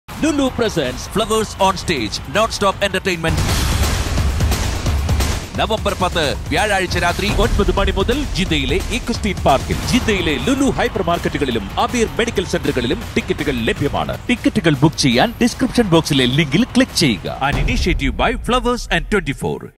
പ്രവാസി മലയാളികളുടെ സ്വർഗ നഗരങ്ങളിൽ മണൽക്കാറ്റ് പോലും മാറി നിൽക്കുന്ന താര ചന്ദ്രോത്സവം രാവ്
'ഫ്‌ളവേഴ്‌സ് ഓൺ സ്റ്റേജ്' നവംബർ 10 വ്യാഴാഴ്ച രാത്രി 9 മണി മുതൽ ജിദ്ദയിലെ ഇക്വെസ്‌ട്രീൻ പാർക്കിൽ

FLOWERS ON STAGE
Date: 10th November 2022
Time: 9.00 PM
Venue: EQUESTRIAN PARK, JEDDAH